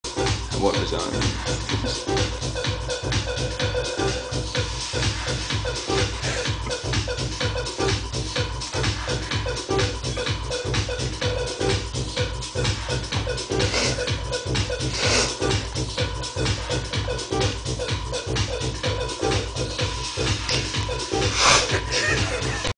anteater
an anteater enjoying his meal